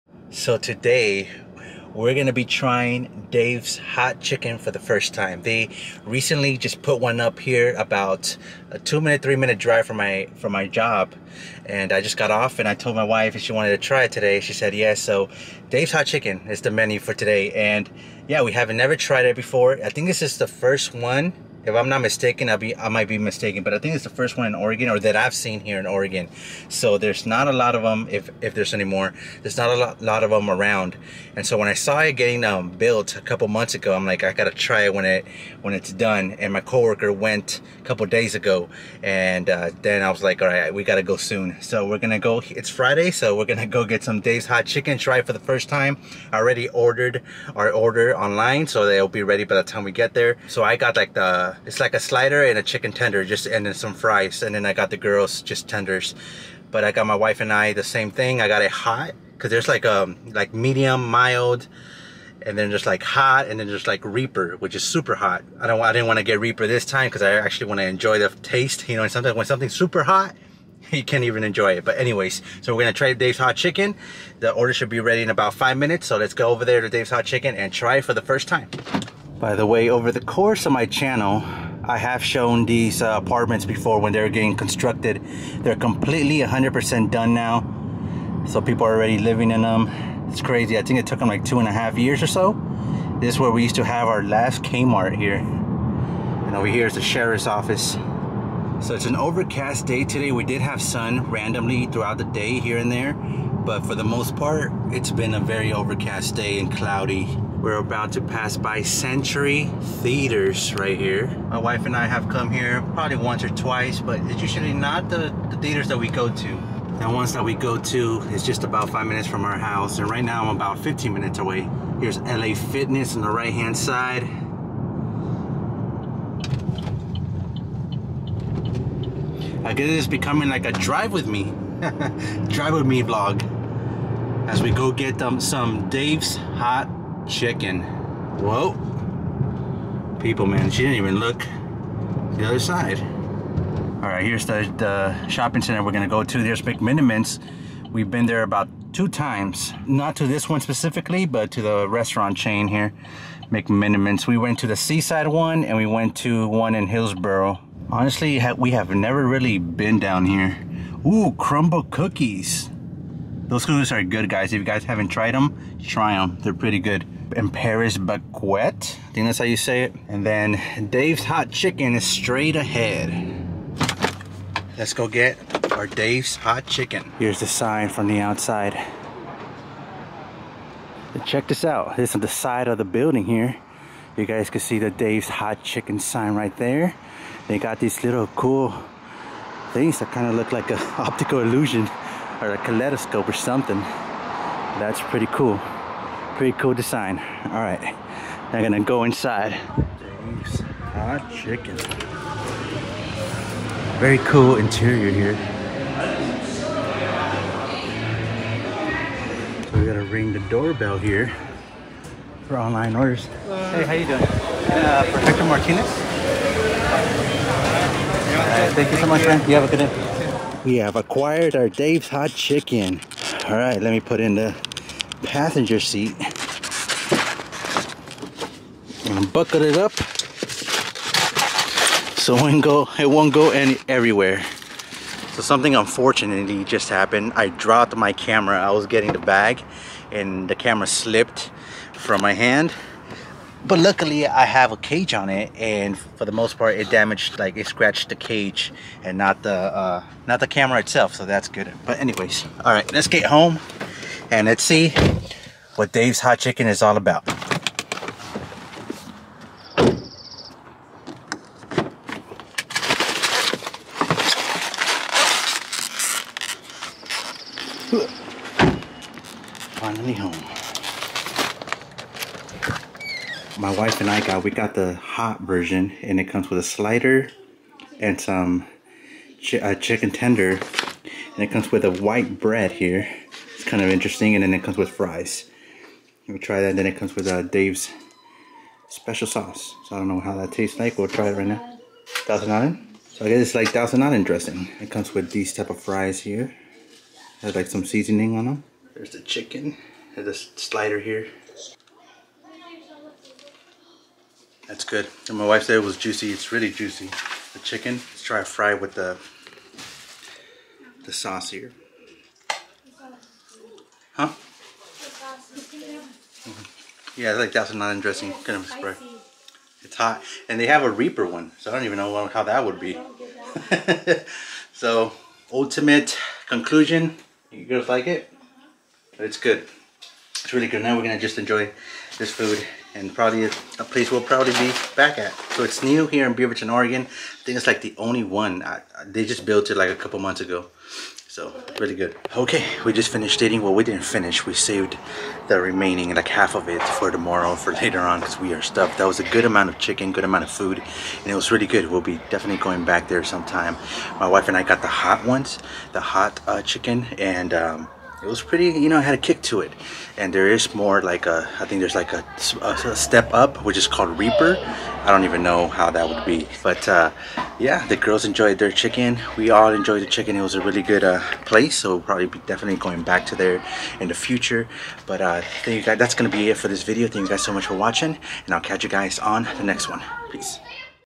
Trying Dave's Hot Chicken For The FIRST TIME!
In todays vlog I try Dave's Hot Chicken For the First Time. They recently opened a Dave's Hot Chicken Here in Beaverton, Oregon we decided to give it a try! It was really good! My wife and I got the "Hot" flavor chicken and it definitely had a kick to it but was delicious! The chicken was very moist.

Camera Gear:
Sony A7C
Sony FE 20mm 1.8 G Lens
Sony ZV-1

Microphones:
Deity D4 Duo

Editing Software:
Video Editing: Final Cut Pro X
Photo/Thumbnail Editing: Adobe Photoshop/Lightroom